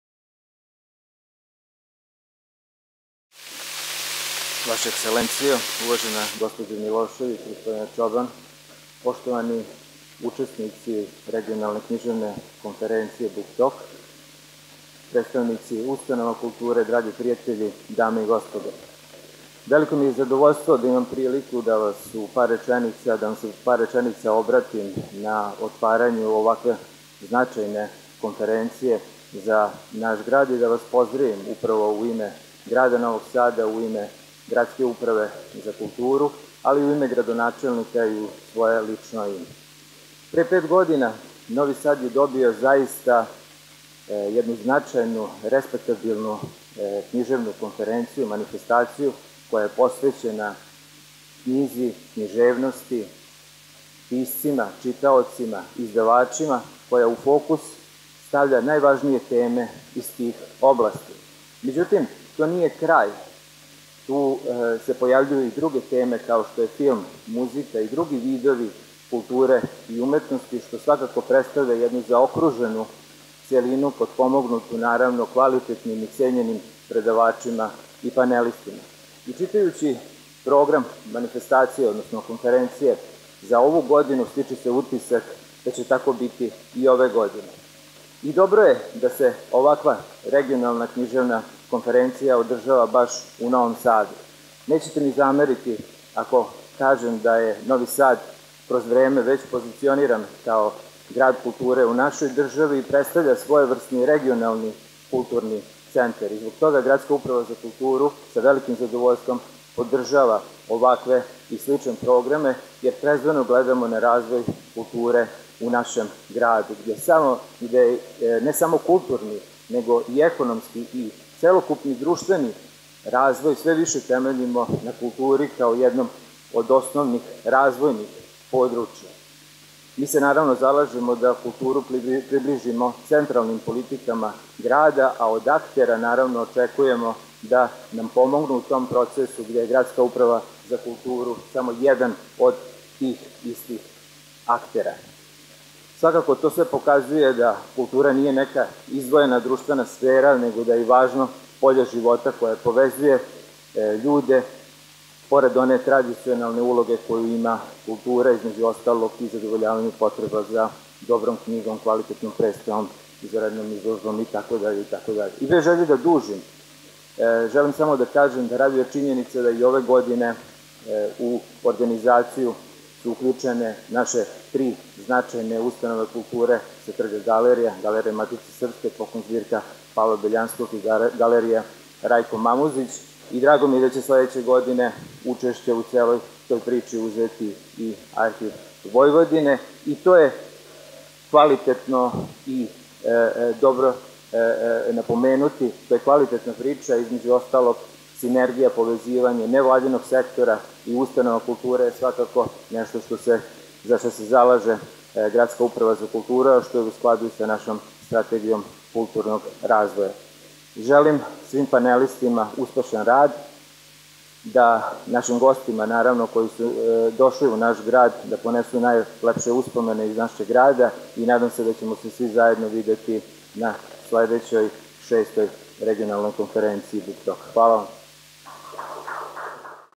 Book Talk 2019 - Dalibor Rožić - otvaranje konferencije
Otvaranje konferencije - Dalibor Rožić, član Gradskog veća za kulturu